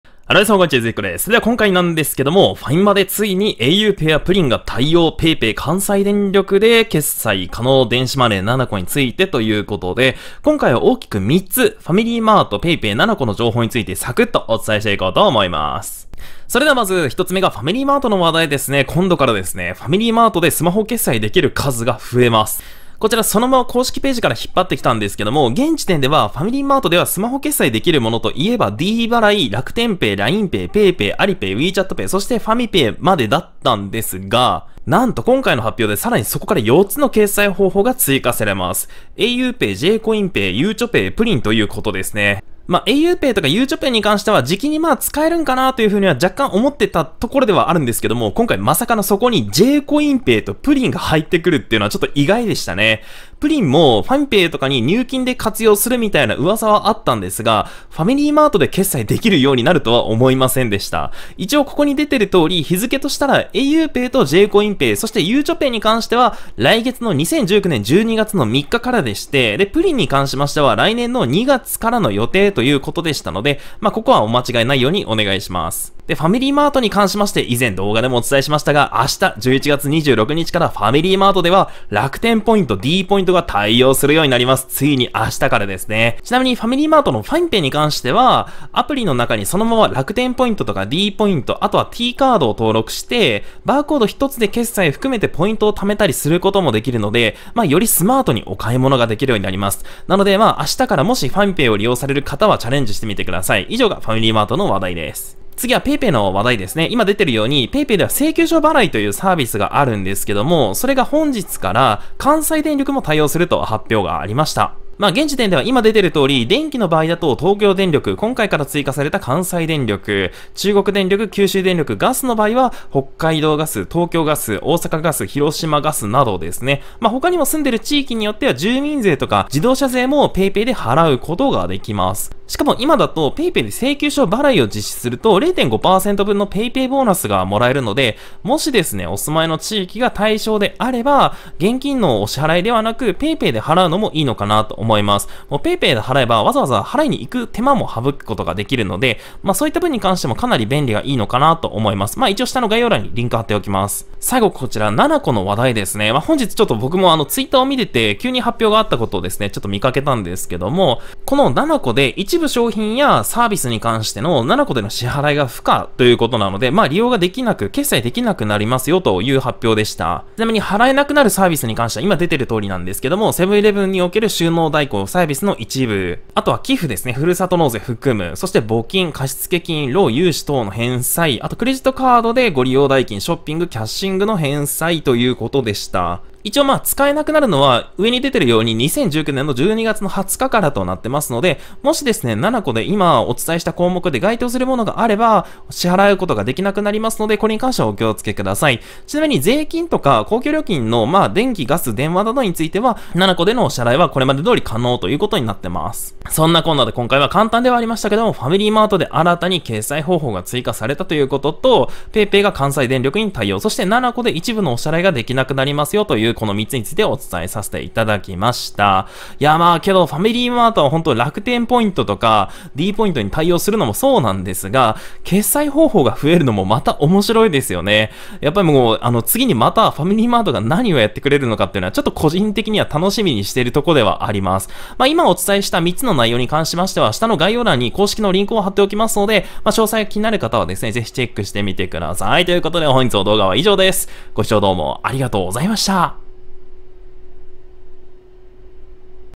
ファミマで遂にauPAYやpringが対応・PayPay関西電力で決済可能・nanacoで一部支払い不可に
【PR】 ファミマ×「au　PAY」、「J‐Coin Pay」、「ゆうちょPay」、「pring」導入で11種類に

使用機材
マイク：Rode NT1A
キャプチャーボード：I-O DATA GV-USB3
オーディオインターフェース：YAMAHA AG03